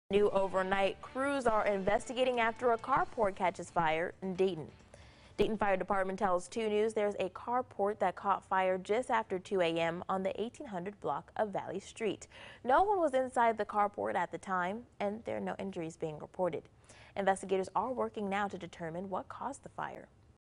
Carport catches fire in Dayton
Carport heavily damaged in fire